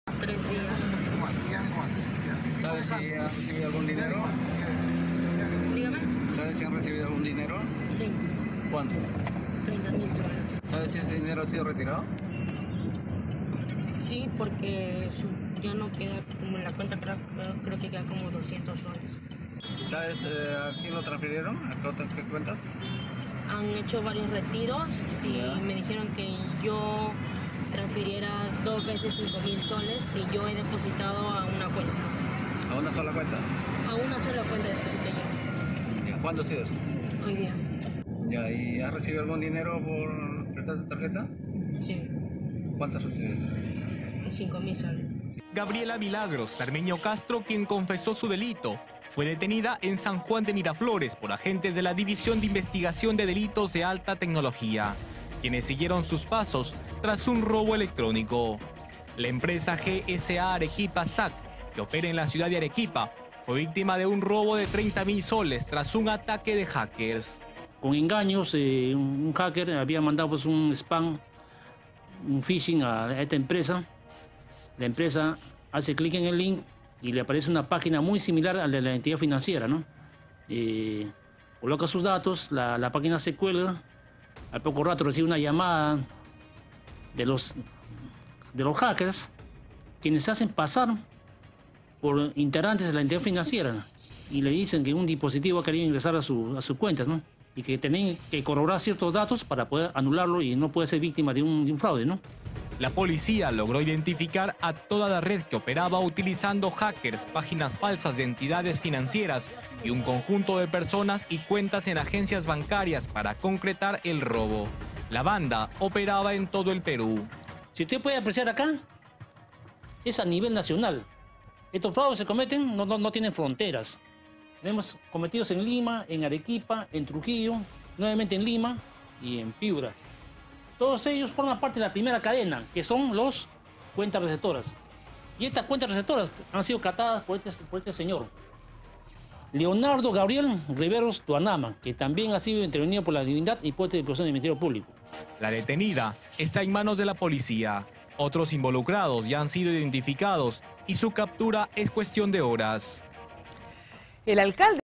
SJL: detienen a mujer implicada en robo informático de S/30 mil a empresa
Agentes de la División de Investigación de Delitos de Alta Tecnología detuvieron a Gabriela Milagros Tarmeño Castro quien participó del robo informático de 30 mil soles a una empresa.

La mujer que fue detenida en el distrito de San Juan de Lurigancho confesó ante los agentes el robo a la empresa G.S.A Arequipa SAC, que opera en la ciudad de Arequipa, tras un ataque de jakers.

La policía identificó a toda la red que operaba utilizando jakers, páginas falsas de entidades financieras y cuentas en agencias bancarias para concretar el robo, la banda operaba en todo el Perú.

CUESTIÓN DE HORAS PARA DETENER A CÓMPLICES

La detenida está en manos de la Policía, otros involucrados ya han sido identificados y su captura es cuestión de horas.